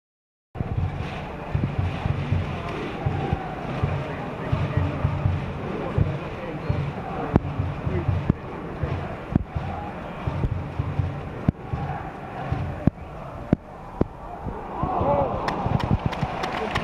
Leicester City vs Manchester City - Leicester free kick, Robert Huth header
Leicester City vs Manchester City - First half Leicester free kick, Robert Huth header over bar. Taken by Fuchs